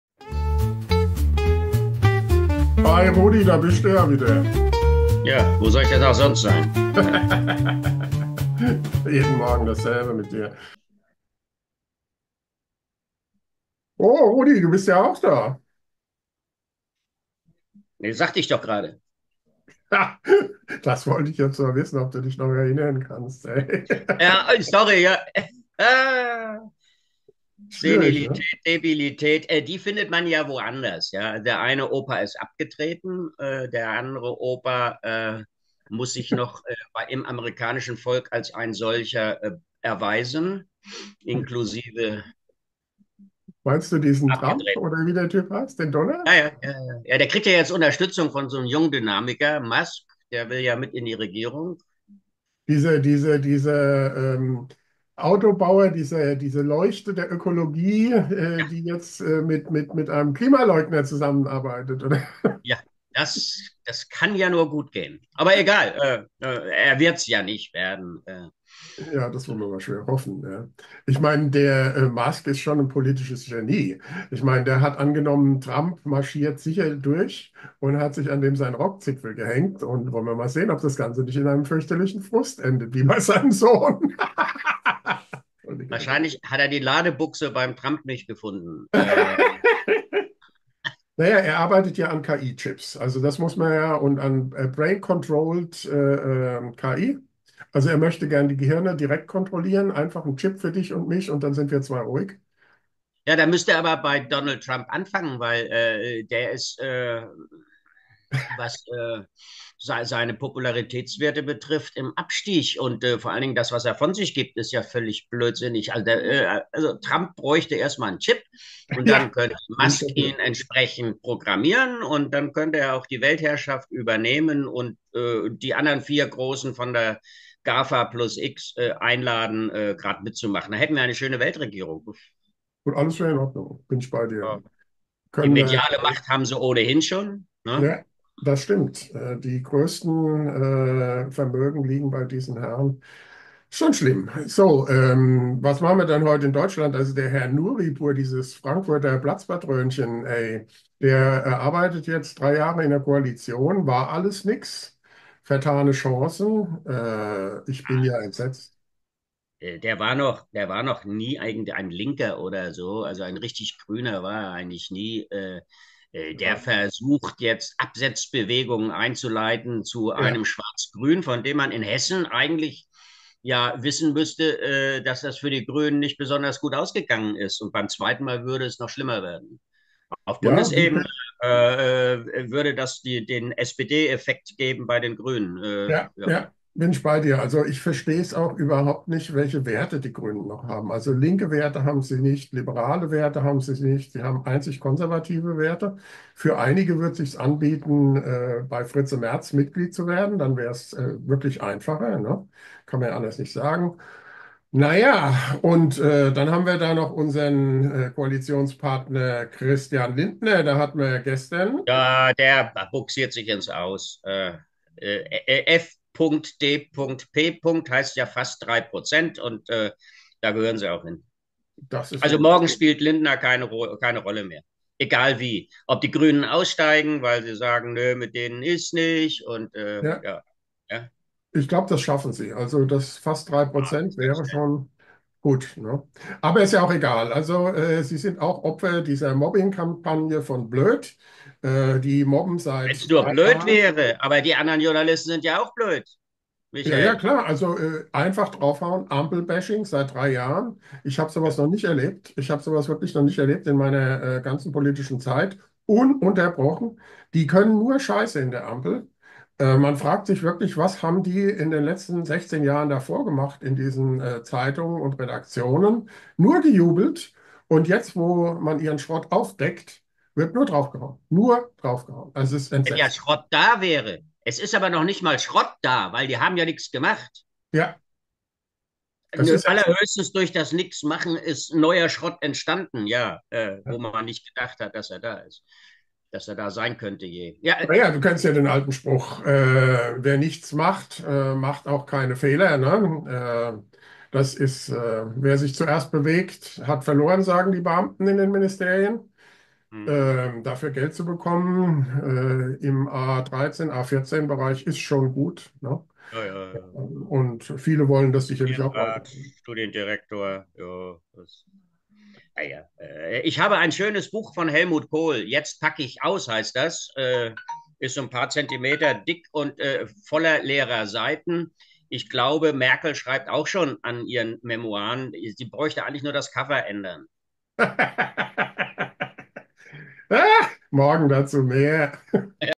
X MIN Trump kommt 20082024
Trump kommt!
Nouripour allein zu Hause
Hat Lindner Zukunft?
Kommt Kohl zurück?